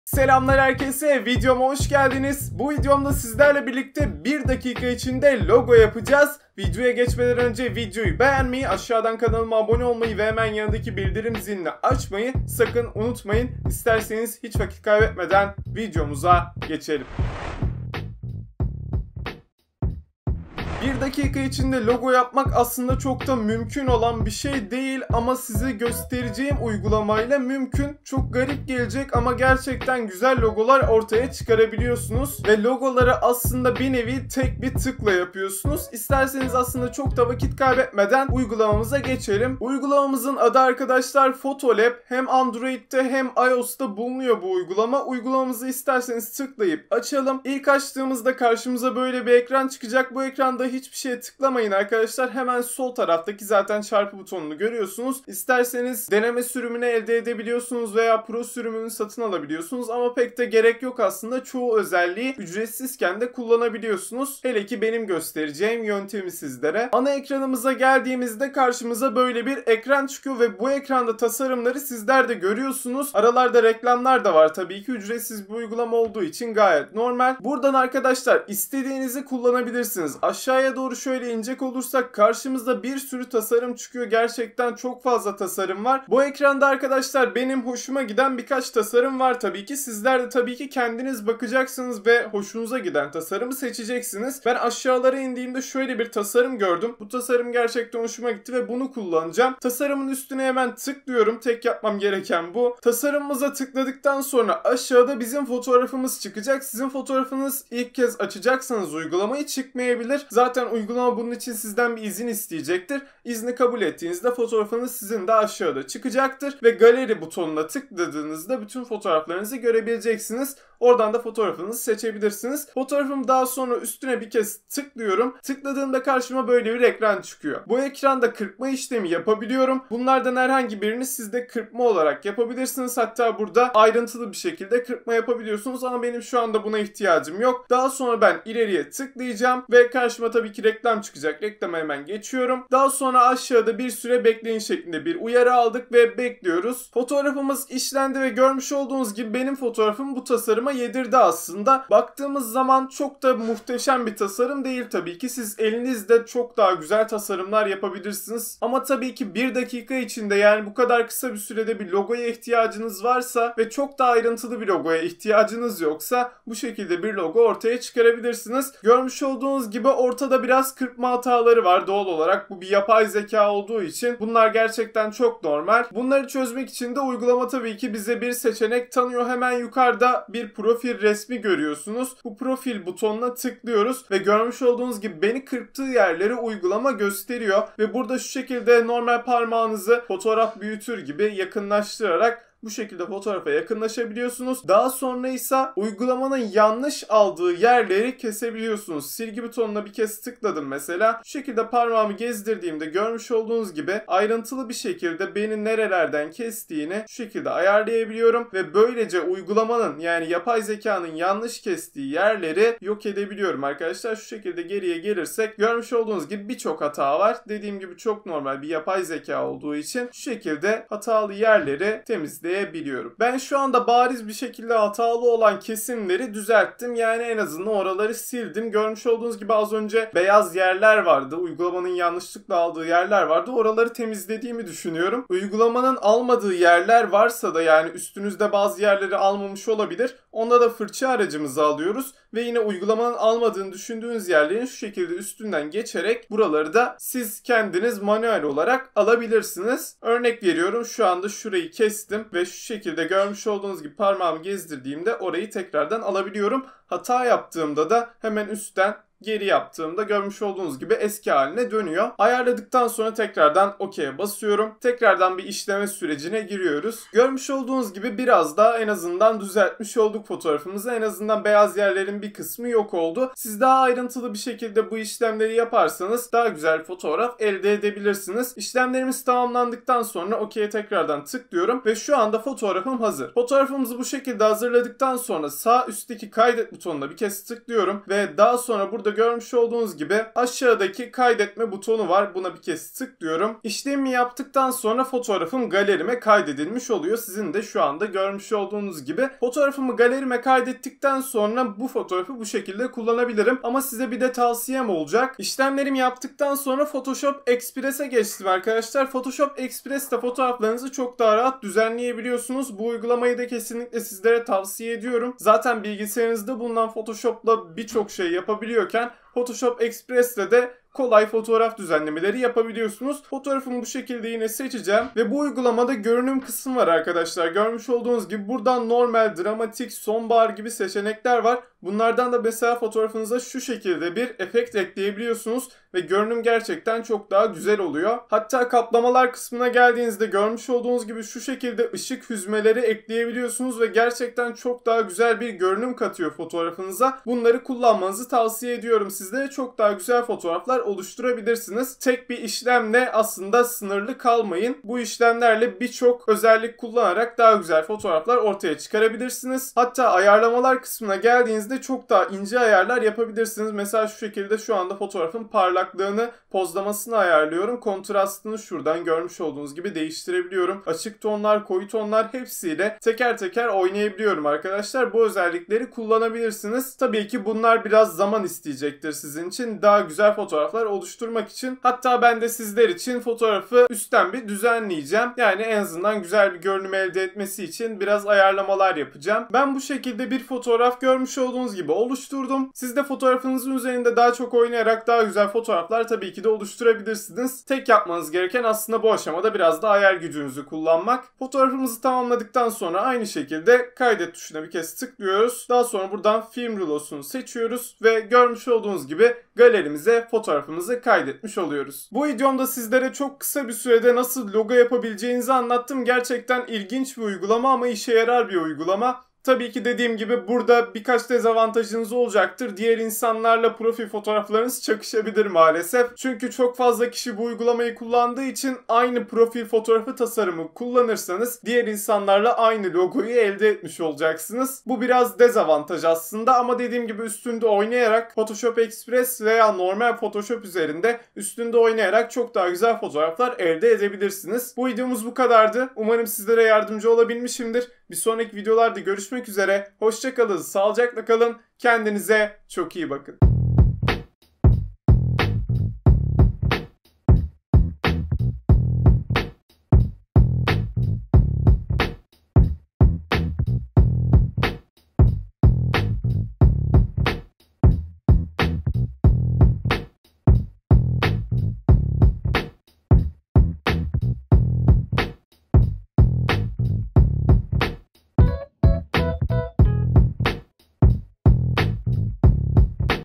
TEK TIKLA YOUTUBE KANAL LOGOSU NASIL YAPILIR? - Youtube Logo Yapımı
Bu videoda sizlere tek tıkla Youtube kanal logosu yapabileceğiniz bir uygulamayı tanıttım. Logo nasıl yapılır? Youtube profil resmi nasıl yapılır? sorularının cevapları videomda. İyi seyirler!